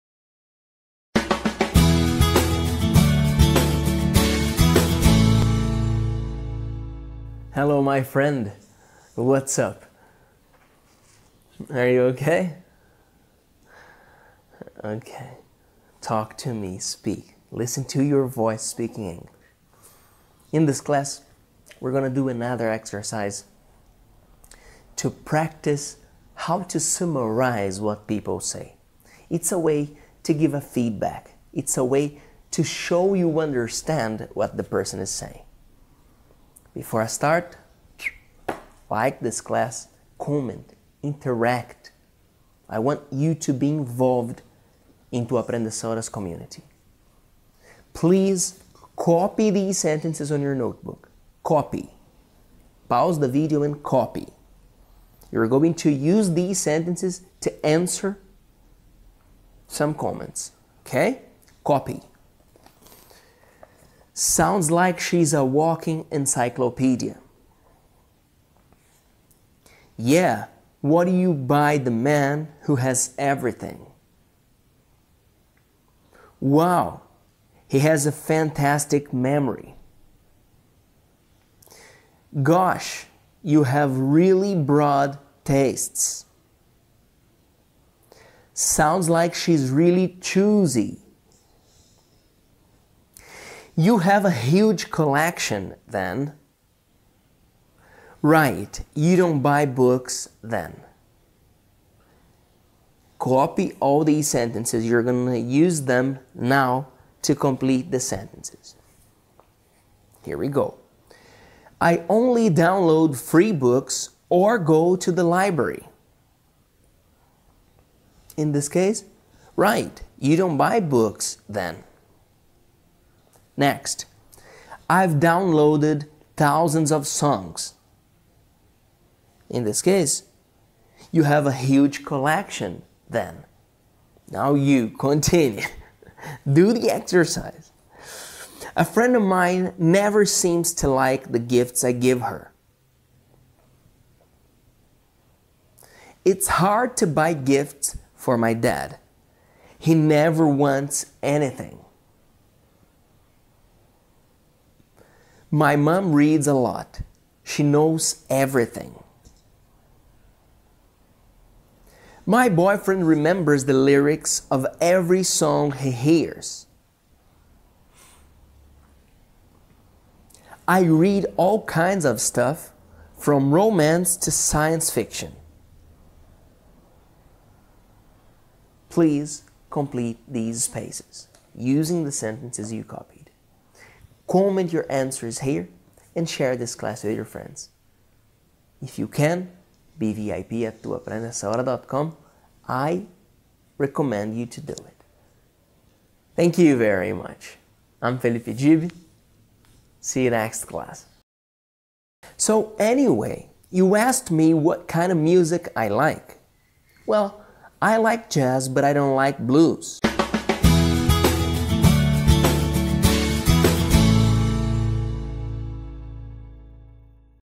CLASE DE INGLÉS 889 How to summarize ideas part 3
Clase del curso de inglés tú aprendes ahora how to summarize ideas part 3 (como resumir ideas parte 3 ). Talk to me (háblame). Listen to your voice speaking English (escucha tu voz hablando inglés). An exercise to practice how to summarize what people say (un ejercicio para practicar como resumir lo que las personas dicen). A way to give a feedback (una forma de dar un feedback). A way to show you understand what the person is saying (una forma de demostrar que entiendes los que la persona esta diciendo). "Like" this class (dale "me gusta" a esta clase). Interact (interactúa). I want you to be involved in Tú Aprendes Ahora's community (quiero verte involucrado en la comunidad de Tú Aprendes Ahora). Copy these sentences on your notebook (copia estas oraciones en tu cuaderno). Pause the video and copy (pausa el video y copia). Use these sentences to answer some comments (usa estas oraciones para responder algunos comentarios). Copy (copia). Sounds like she's a walking encyclopedia (suena que ella es una enciclopedia andante). Yeah. What do you buy the man who has everything? (sí. ¿qué le compras al hombre que tiene todo?). Wow. He has a fantastic memory (wow. él tiene una fantástica memoria). Gosh. You have really broad tastes (dios. Tienes gustos realmente amplios). Sounds like she's really choosy (suena que ella es realmente exigente). You have a huge collection, then (tienes una enorme colección, entonces). Right. You don't buy books, then (bien. no compras libros, entonces). Use these sentences to complete the "___" (usa estas oraciones para completar el "___"). I only download free books or go to the library. ___ (solamente descargo libros gratis o voy a la biblioteca. Right. You don't buy books, then (bien, no compras libros, entonces). I've downloaded thousands of songs. ___ (he desacragado miles de canciones. You have a huge collection, then (tienes una gran colección, entonces). Do the exercise (haz el ejercicio). A friend of mine never seems to like the gifts I give her. ___ (a una amiga mía nunca parece gustarle los regalos que le doy.___). It's hard to buy gifts for my dad (es díficil comprar regalos para mi papá). He never wants anything. ___ (él nunca quiere nada. My mom reads a lot (mi mamá lee mucho). She knows everything. ___ (ella sabe todo. My boyfriend remembers the lyrics of every song he hears. ___ (mi novio recuerda la letra de cada canción que escucha). I read all kinds of stuff from romance to science-fiction. ___ (leo todo tipo de cosas, desde romance hasta ciencia ficción. Complete these "___" using the sentences you copied (completa estos "___" usando las oraciones que copiaste). Comment your answers here and share this class with your friends (comenta tus respuestas aquí y comparte esta clase con tus amigos). I recommend you to do it (te lo recomiendo). See you next class!